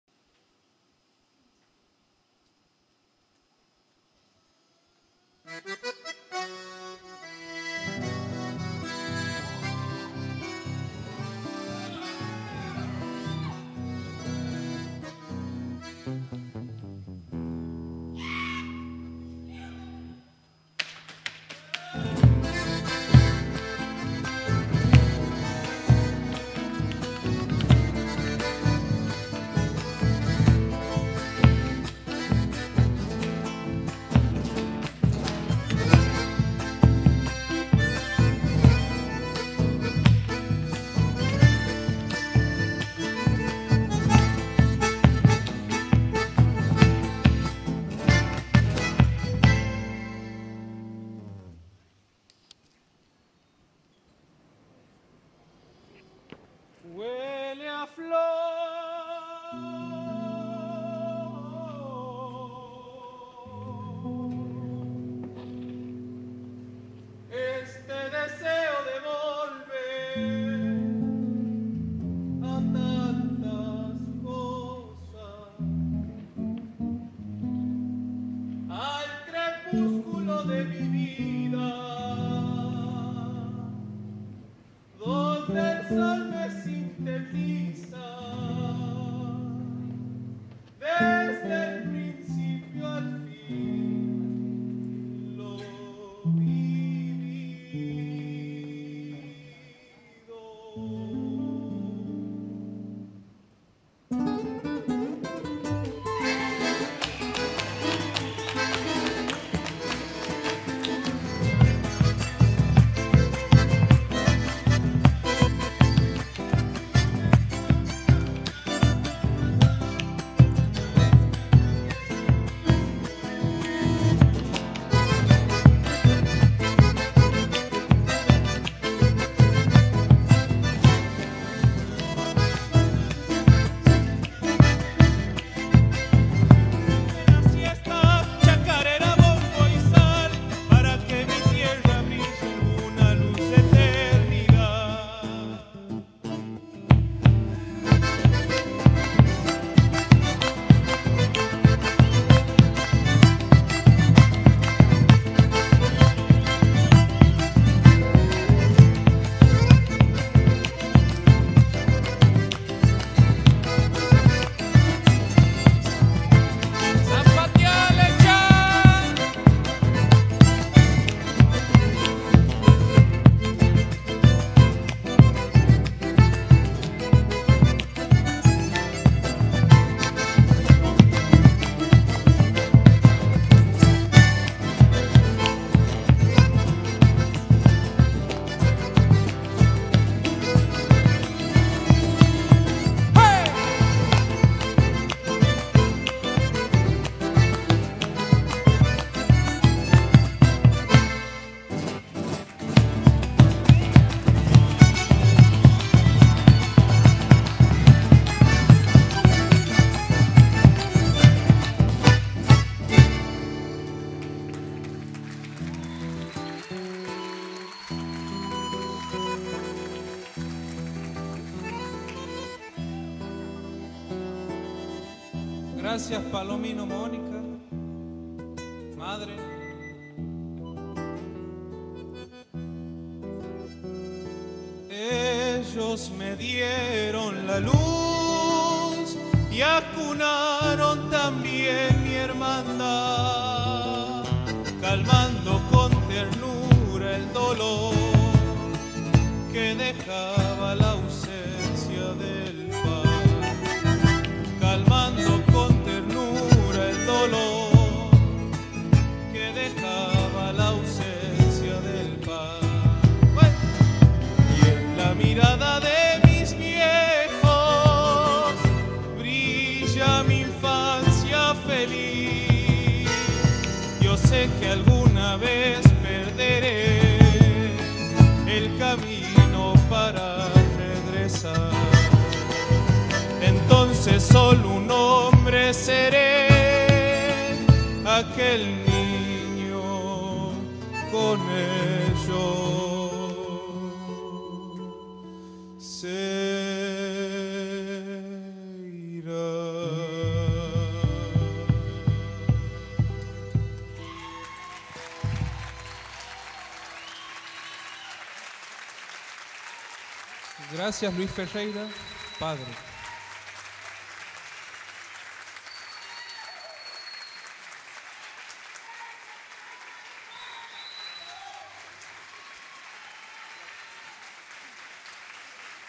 Nicolas Ferreyra en el Teatro 25 de Mayo
Canta Octavio Muratore
Musicos Enzo Luna, Ivan Coria, Raul Toscano, Dilan Coronel

Actuacion de Nicolas Ferreyra en el recital de Nacidos del Tiempo, realizado en el teatro 25 de Mayo en el ciclo Cultura Popular

Nicolas Ferreyra: primera voz y base
Carlos Leiva: segunda voz y bombo
Santiago Lopez: tercera voz, bajo y arreglos
Nicolas Castillo: bandoneón
Ítalo Banegas: primera guitarra